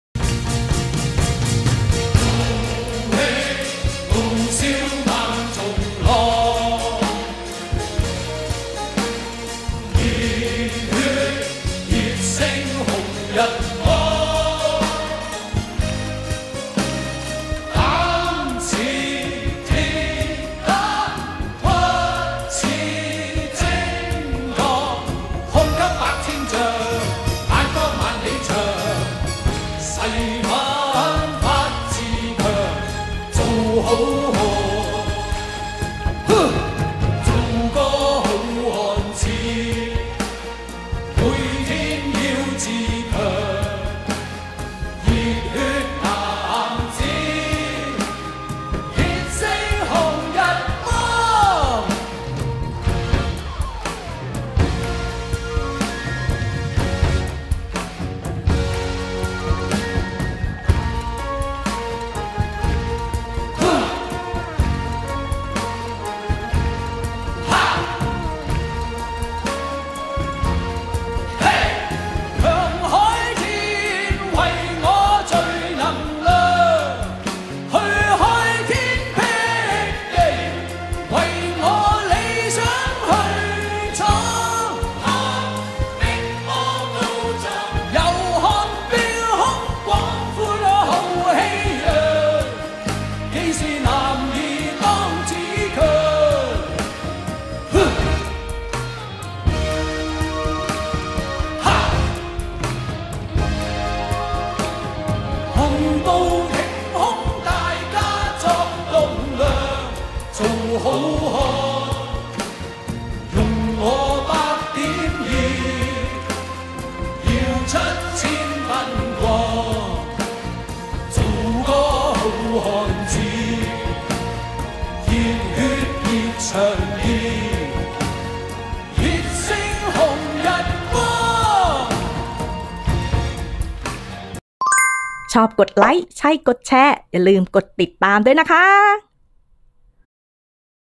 เทศกาลตวนอู่ 5ค่ำเดือน5 ห่อบ๊ะจ่าง 🐲แข่งเรือมังกร 🐲
วันนี้ (10 มิ.ย.2024) ตรงกับ เทศกาลตวนอู่ 5ค่ำเดือน5 ห่อบ๊ะจ่าง 🐲แข่งเรือมังกร 🐲
📌ดูแข่งเรือมังกรจีน…ต้องดูของมณฑลกวางตุ้ง!!!
📌ดูแข่งเรือมณฑลกวางตุ้ง…ต้องดูของ เมืองโฝซาน!